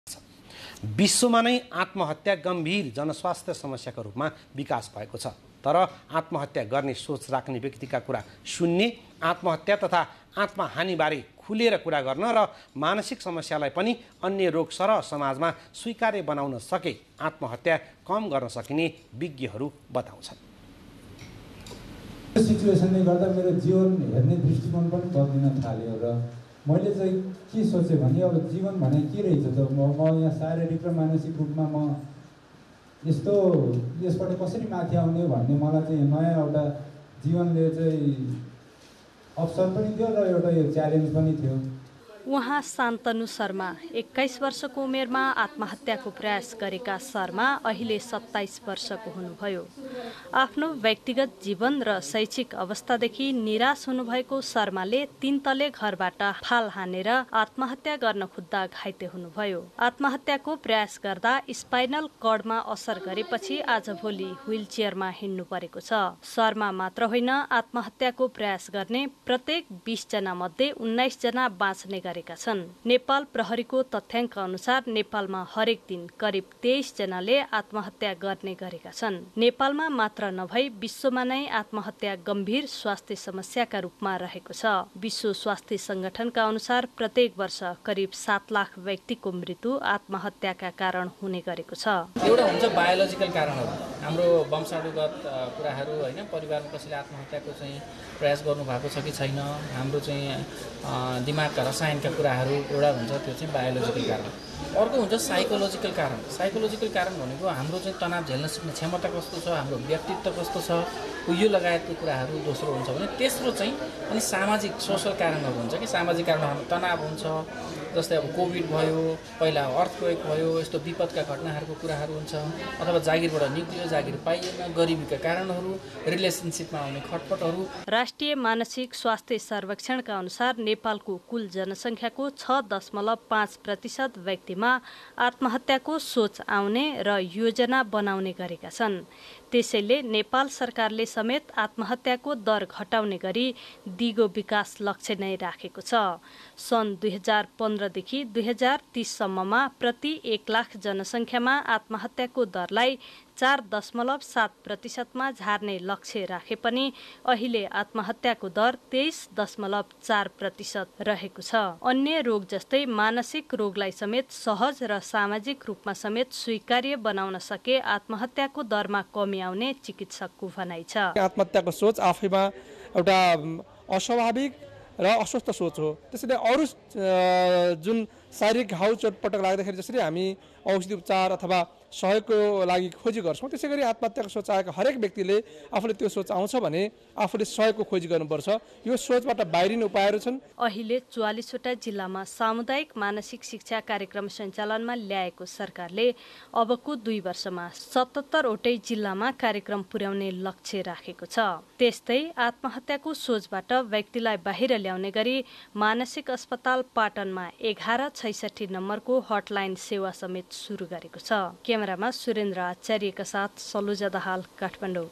विश्वमानै आत्महत्या गम्भीर जनस्वास्थ्य समस्याका रुपमा विकास भएको छ
विश्वमानै आत्महत्या गम्भीर जनस्वास्थ्य समस्याका रुपमा विकास भएको छ । तर आत्महत्या गर्ने सोच राख्ने व्यक्तीका कुरा सुन्ने, आत्महत्या तथा आत्महानी बारे खुलेर कुरा गर्न र मानसिक समस्यालाइ पनि अन्य रोग सरह समाजमा स्वीकार्य बनाउन सके आत्महत्या कम गर्न सकिने विज्ञहरु बताउछन् ।।